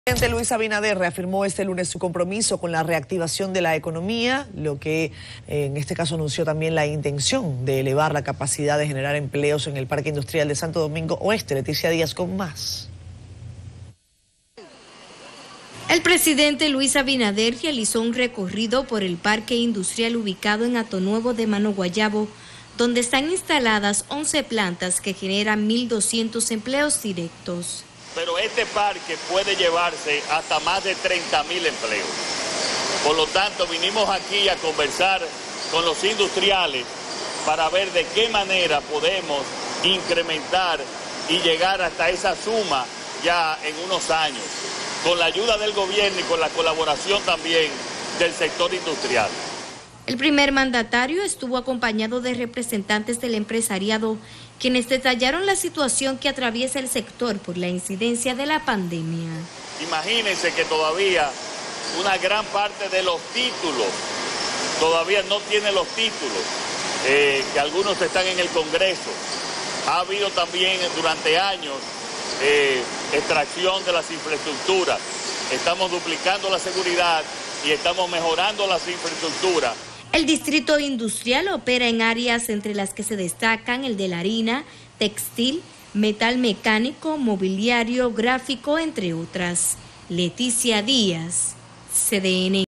Abinader busca elevar a 30 mil empleos de zona industrial Santo Domingo Oeste
El presidente Luis Abinader, reiteró este lunes su compromiso con la reactivación de sectores económicos por lo que anunció la intención de elevar la capacidad de empleomanía en el Parque Distrito Industrial de Santo Domingo Oeste a unos 30 mil trabajadores.